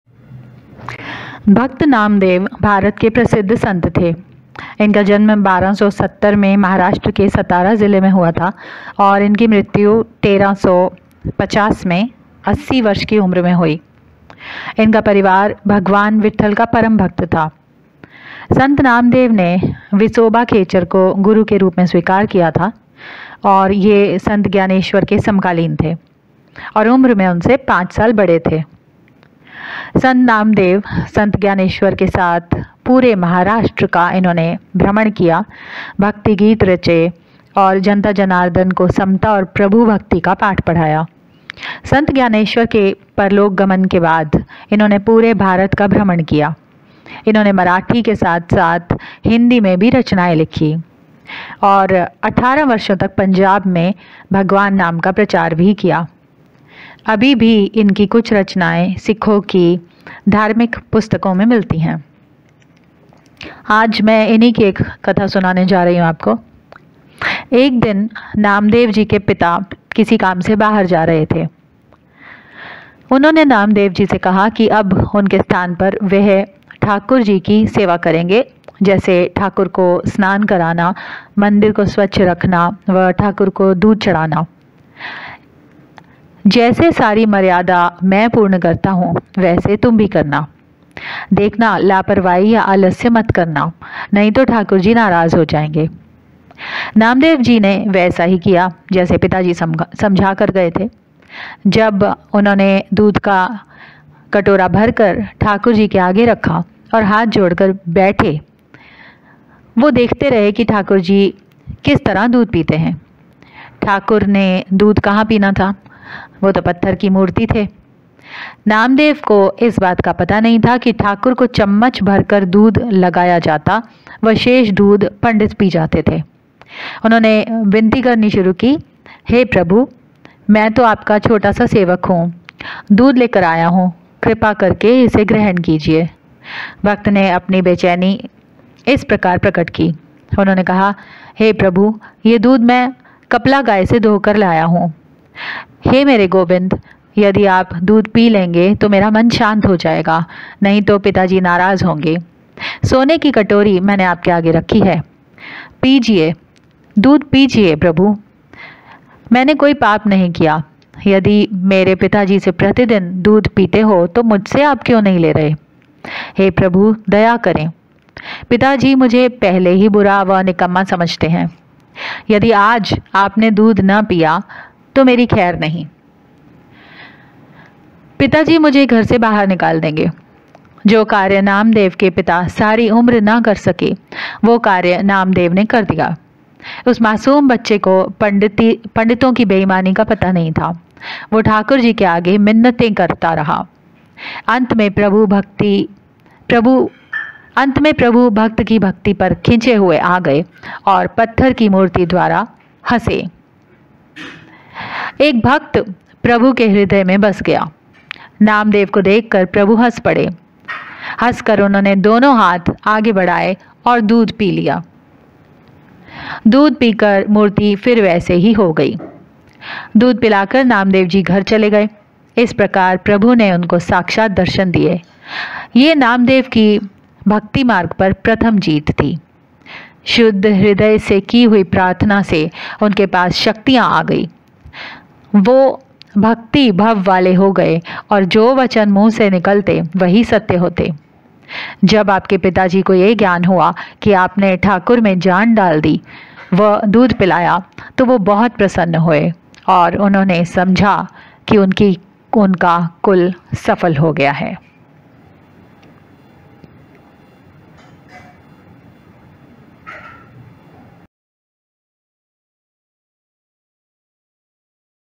संत नामदेव - शुद्ध हृदय से की हुई प्रार्थना से उनके पास शक्तियाँ आ गई
संत नामदेव - शुद्ध हृदय  से की हुई प्रर्थना से उनके पास शक्तियाँ आ गई

Second channel name : Care for animals
Handle :  @Loveandserveanimals
Be kind to animals. Do not kill, torture or eat them.

Neem Karoli Baba in dream